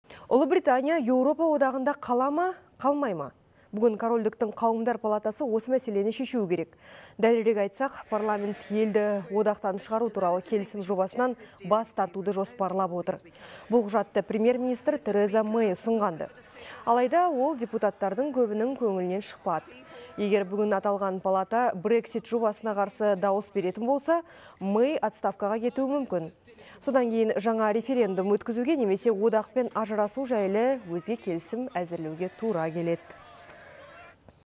Британ парламенті бүгін Еуропа одағынан шығу-шықпау мәселесін шешеді
Ұлыбритания Еуропа одағында қала ма, қалмай ма? Бүгін корольдіктің қауымдар палатасы осы мәселені шешуі керек. Дәлірек айтсақ, парламент елді одақтан шығару туралы келісім жобасынан бас тартуды жоспарлап отыр. Бұл құжатты премьер-министр Тереза Мэй ұсынған-ды. Алайда, ол депутаттардың көбінің көңілінен шықпады. Егер бүгін аталған палата «Брексит» жобасына қарсы дауыс беретін болса, Мэй отставкаға кетуі мүмкін. Содан кейін жаңа референдум өткізуге немесе одақпен ажырасу жайлы өзге келісім әзірлеуге тура келеді.